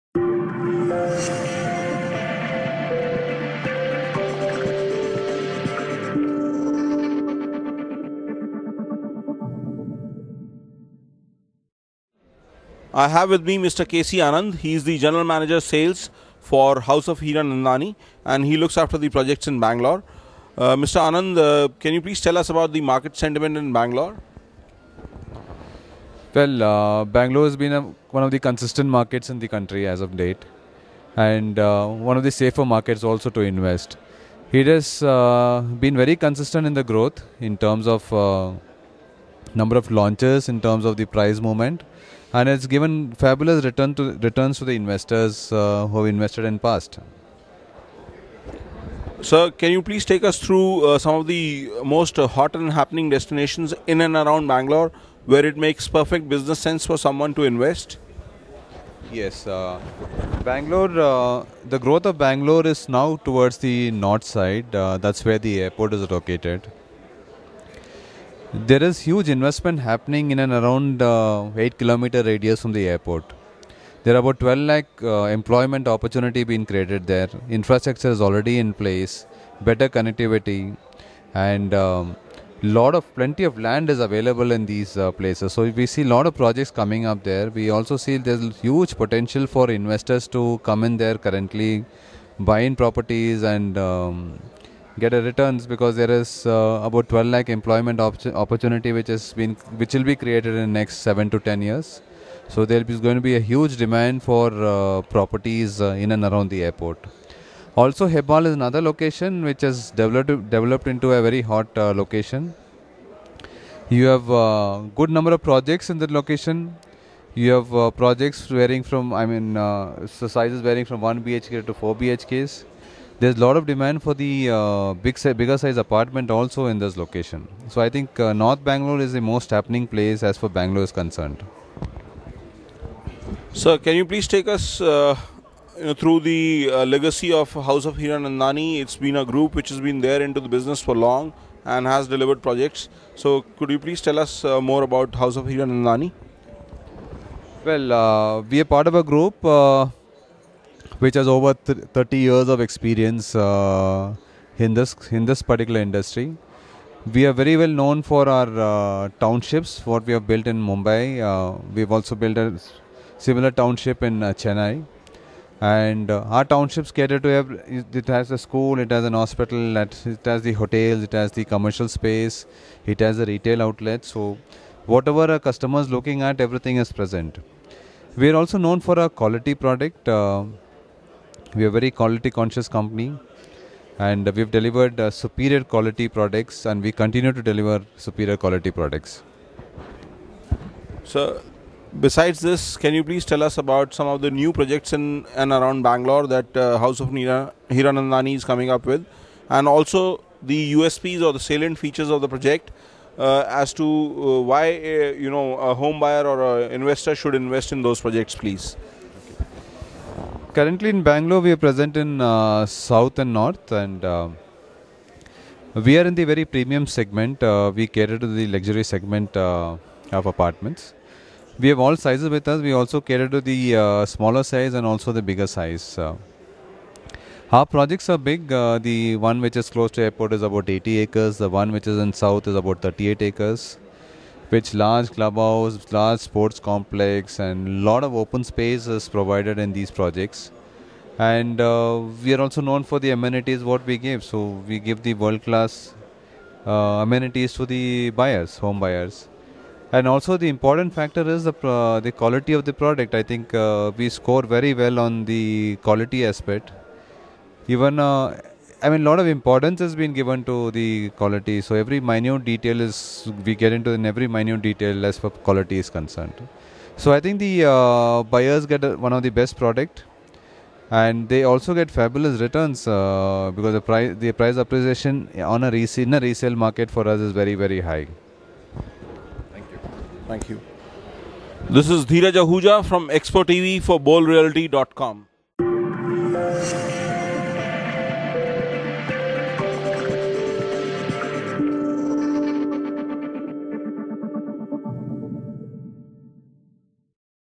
K C Anand, GM-Sales, Heera Nandani speaks to BolREALTY.com
Real Estate Insights. For You. By You.